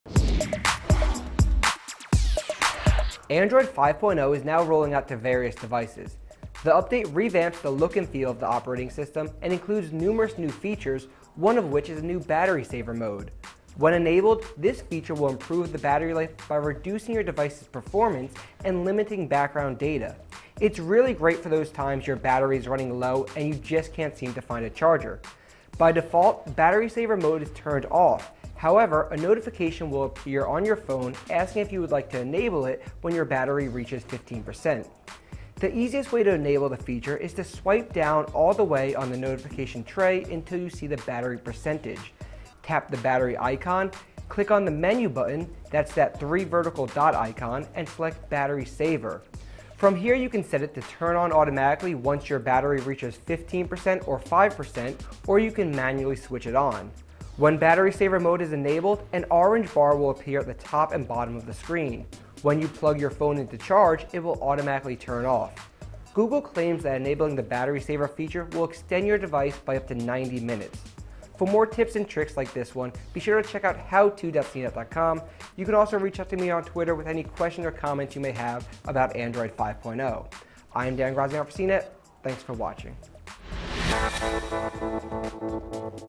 Enable battery saver on Android 5.0 Lollipop
Battery running low and can't seem to find a charger? CNET's Dan Graziano shows you how to extend battery life up to 90 minutes.